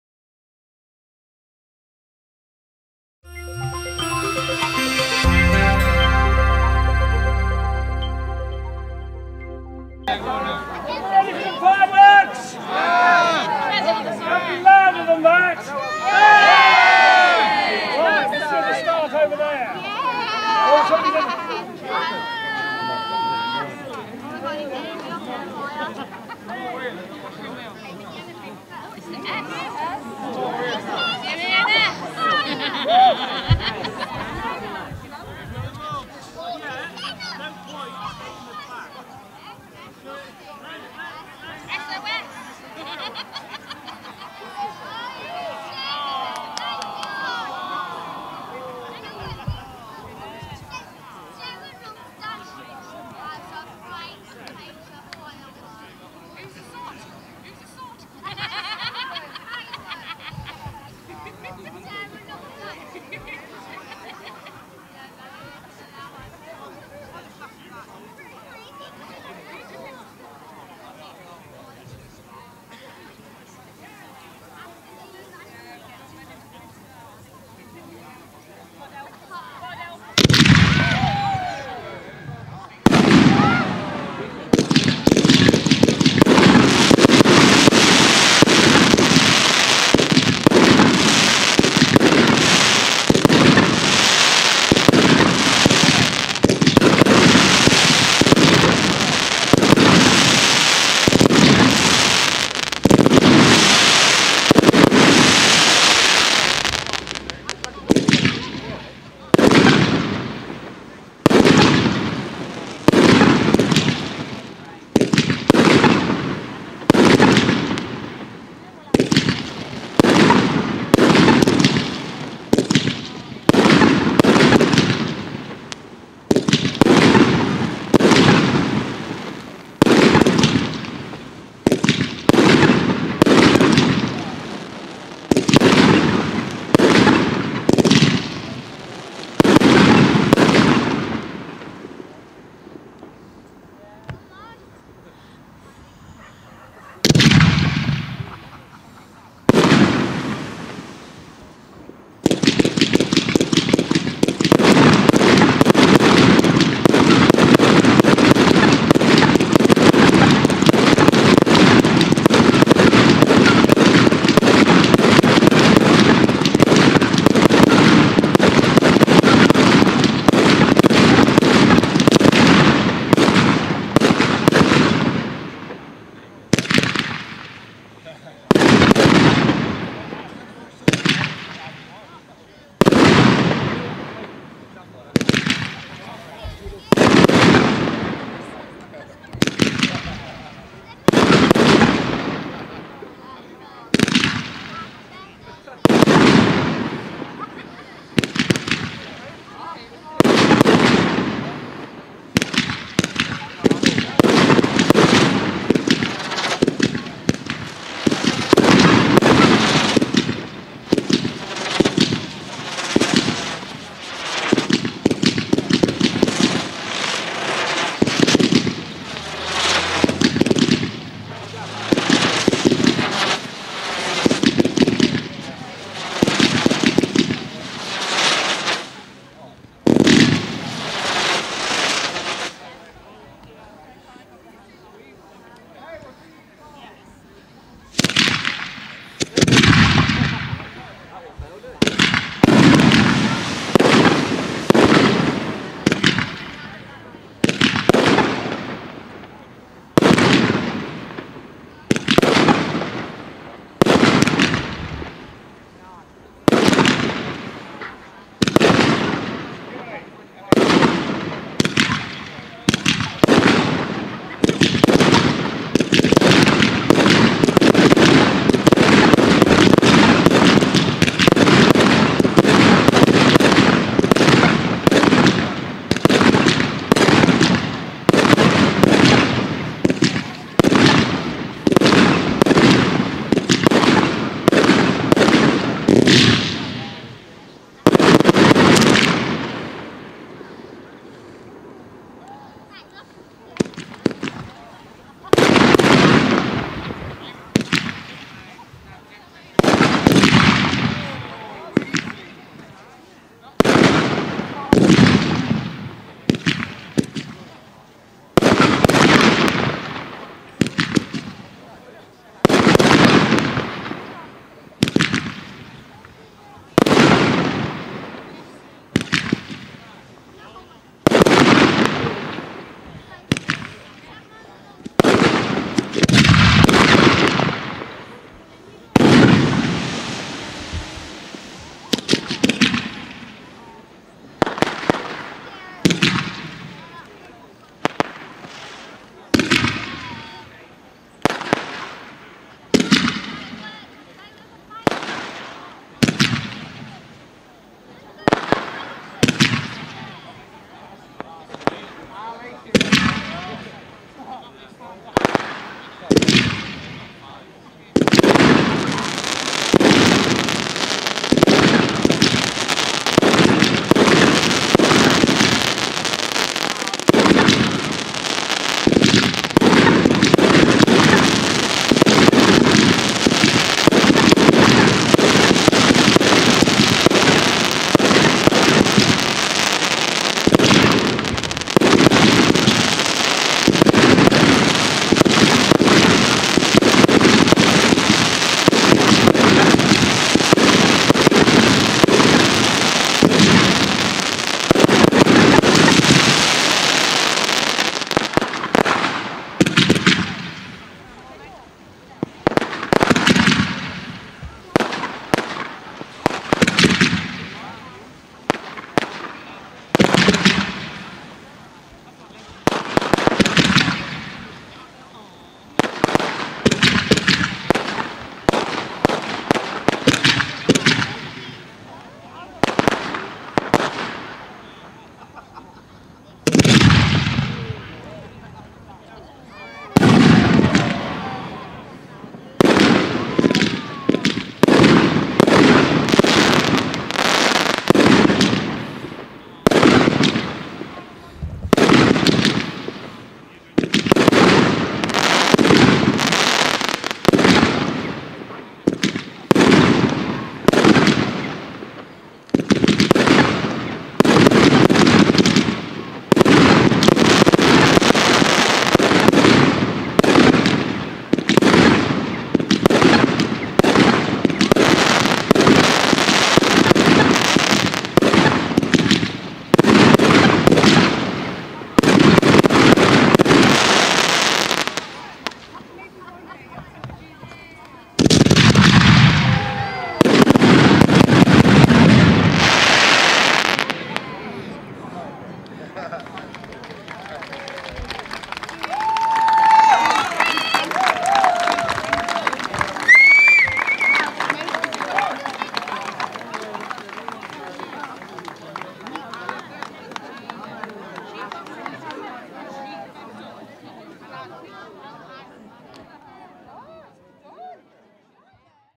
Champagne Display with WSP
This is an example of 1st Galaxy Fireworks Champagne wedding display with a wedding set piece in fire writing. This display can be personalised with your choice of colours, and additions such as the set piece, heart shape shells etc. Contact our expert wedding show planners for a personal quote